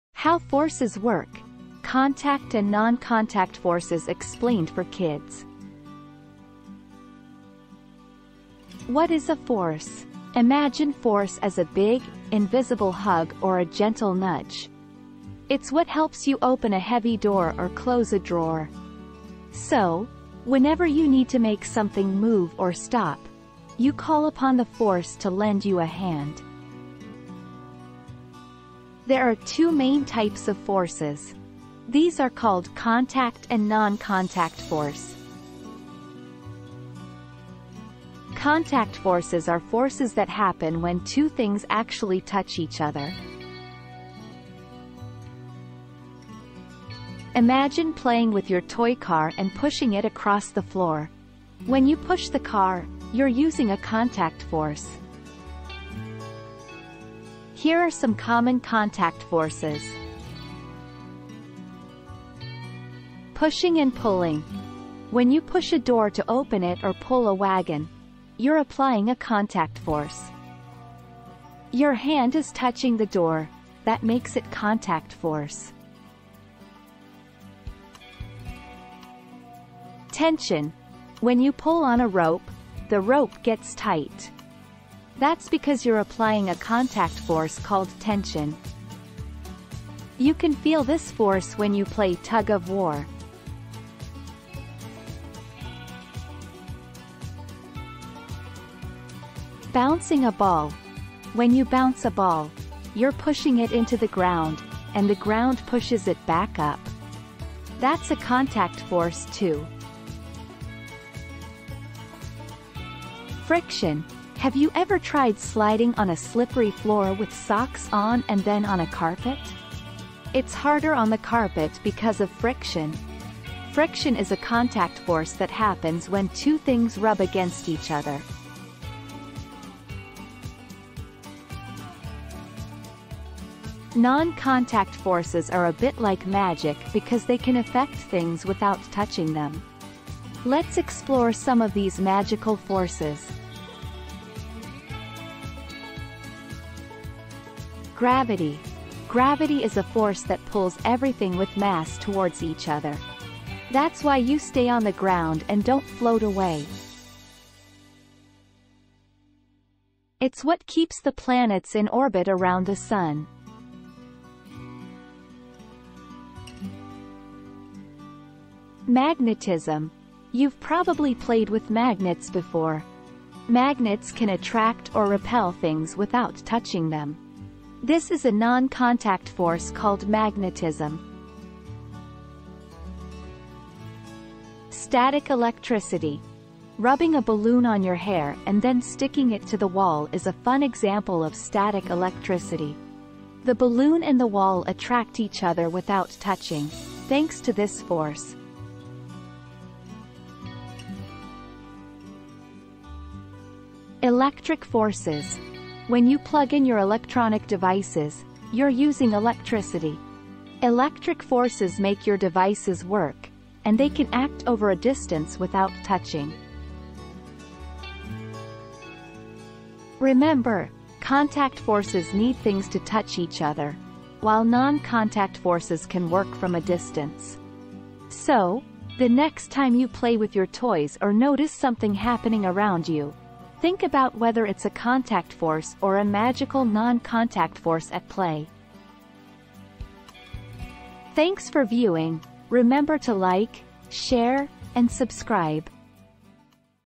What is Force? Contact and Non-Contact Force (Grade 3, 4 & 5) | Science for Kids
This video titled "What is Force? Contact and Non-Contact Force (Grade 3, 4 & 5)' teaches children about force. What is force? Force is a fundamental concept in physics, serving as a powerful driver behind the motion and interaction of objects in our physical world. The two types of force are explained - contact and non-contact force.

Contact forces, also known as direct forces, occur when objects physically touch and interact with one another. These forces can manifest in various forms, such as tension, compression, friction, or normal force. When you push a book across a table or pull a door open, you're experiencing contact forces in action.

Non-contact forces, are often referred to as invisible forces because they act at a distance without any direct physical contact between objects. These forces include gravitational attraction, electromagnetic forces, and magnetic forces. The Earth's gravitational pull on objects, the repulsion or attraction of charged particles, and the behavior of magnets all exemplify non-contact forces at work.

This video is filled with examples of contact and non-contact force. Your Grade 3, 4 & 5 children will love it.